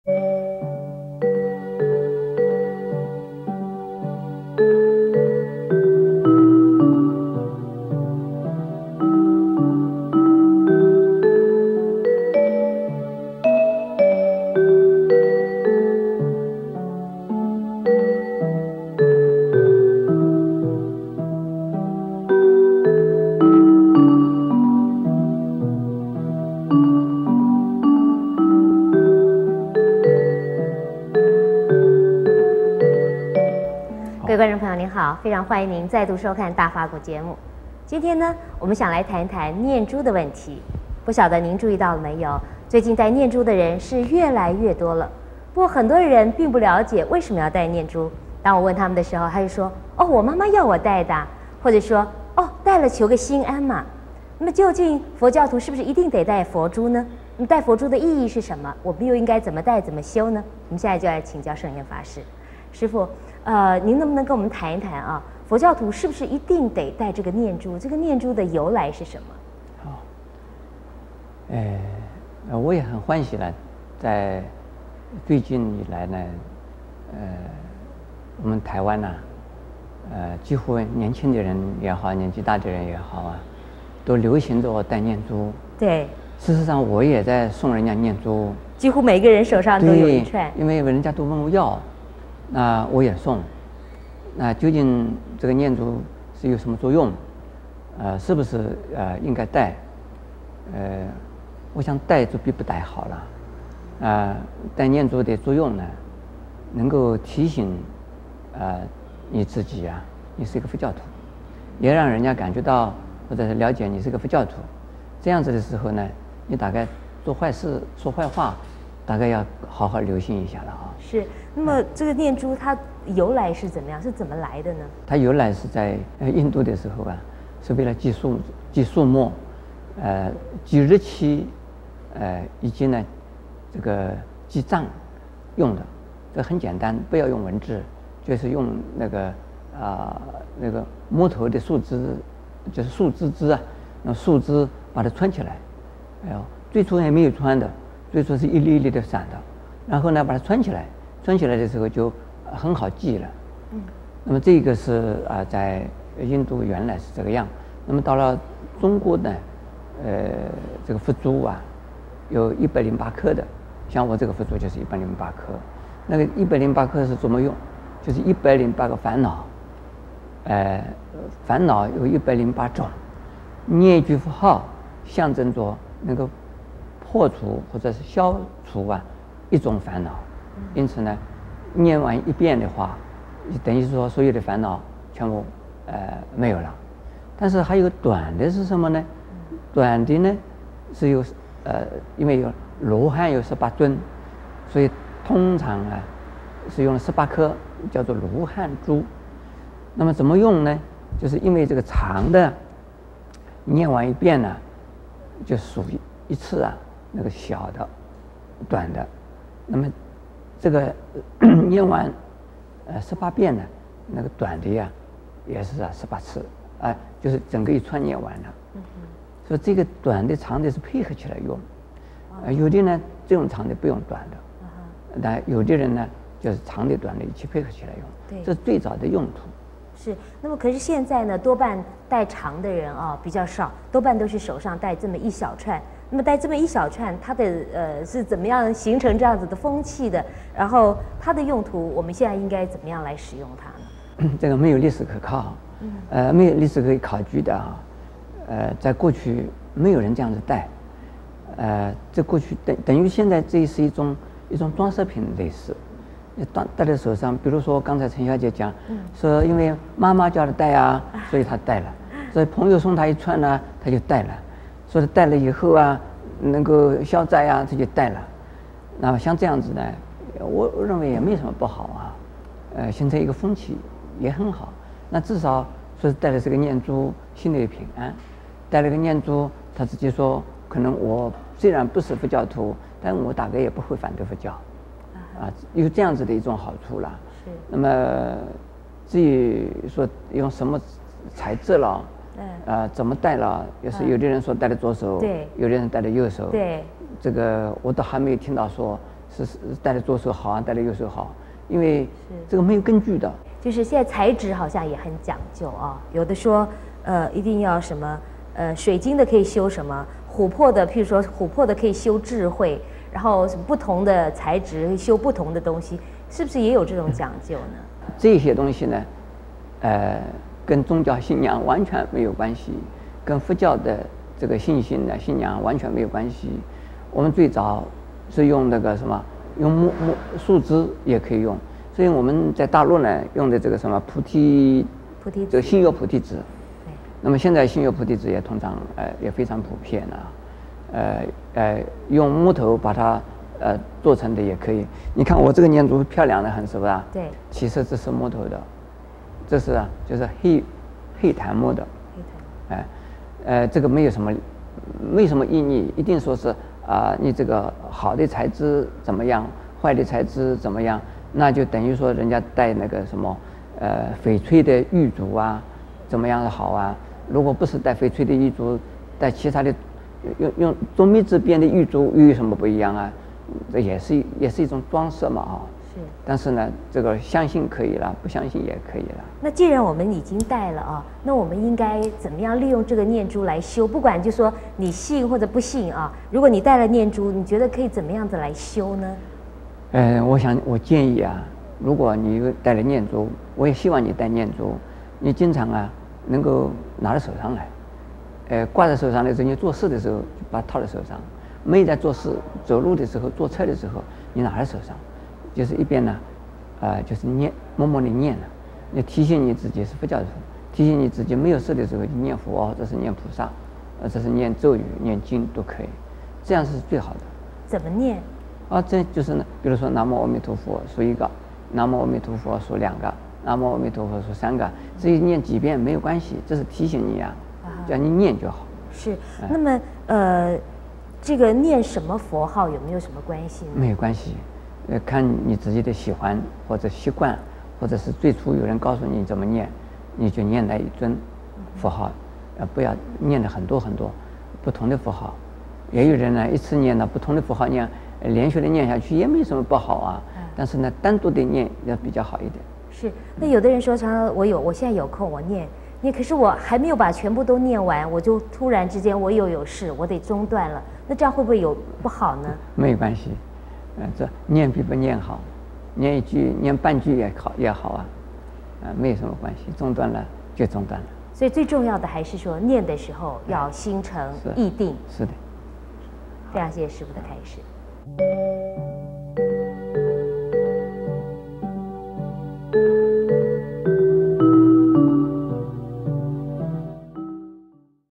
What does it mean to wear Buddha beads?(GDD-51 Master Sheng Yen)
Wearing Buddha beads reminds one to pay attention to one's daily actions. Reciting the Buddha's name signifies eliminating one's vexation. One can recite as many times as one wishes but the important thing is to focus while one is reciting.